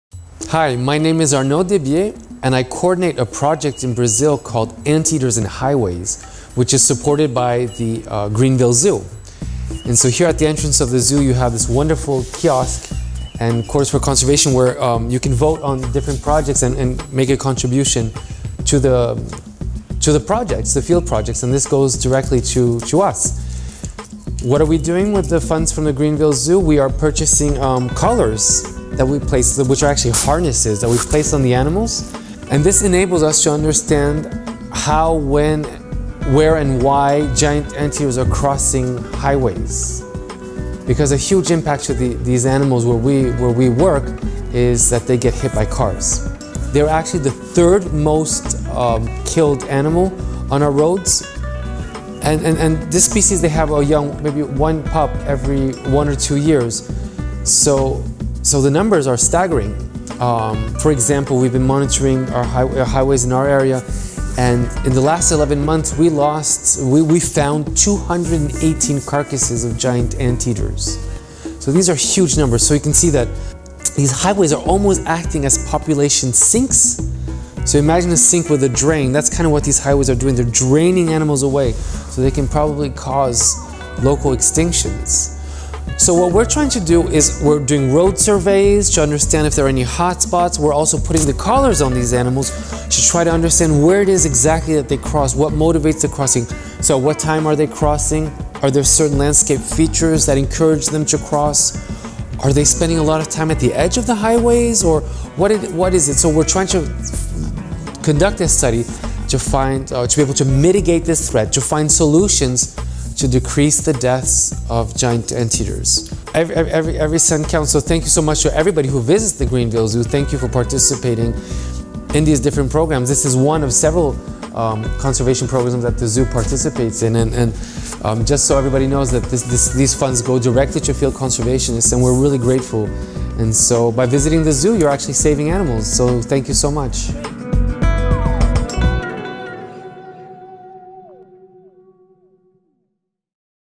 Help the Greenville Zoo Protect Giant Anteaters
The giant anteater is an iconic South American mammal listed as vulnerable to extinction by the International Union for Conservation of Nature (IUCN). The forests and grasslands of the Brazilian Cerrado biome have been strongholds for giant anteaters; however, during the last 35 years, more than half of the Cerrado has been converted into pasture and agricultural lands. The remaining habitat has been dissected by an ever-increasing network of roads and highways, which are inflicting significant damage on the giant anteater population. In fact, the giant anteater is now among the species most killed on roadways in the Cerrado.

To address the threat, ANTEATERS & HIGHWAYS launched a project to collect anteater road interaction data and help protect the species in the wild. Specifically, the research is designed to quantify the impact of roads on giant anteater populations and evaluate their effect on the species’ behavior, population structure, and health. The project team is also investigating whether roads in the Cerrado are affecting anteater population persistence by acting as population sinks and barriers to gene flow.

Through our Quarters for Conservation program, you can make a difference. With the collection of quarters, the zoo and local community provide much-needed funding for Anteaters and Highways as well as other wildlife conservation projects in our region and around the globe.